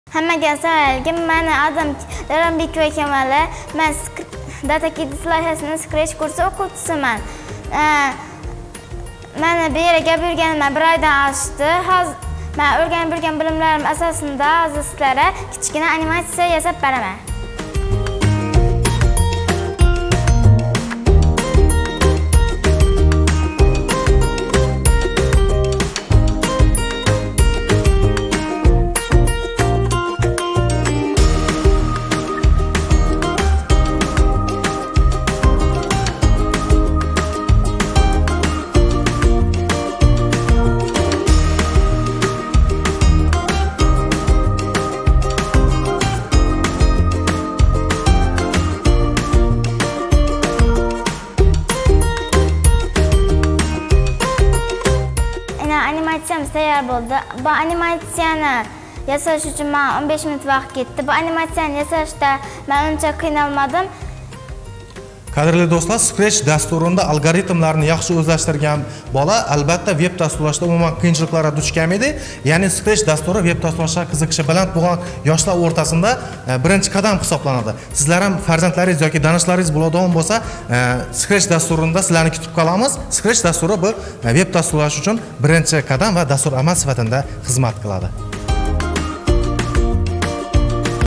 🐱 «Scratch» курси ўқувчиси Камола Давронбекова жуда чиройли анимация яратди
🤩 Анимация 25 хил рангда акс этади

⏱ Бу анимация учун Камола 15 дақиқа вақт сарфлади холос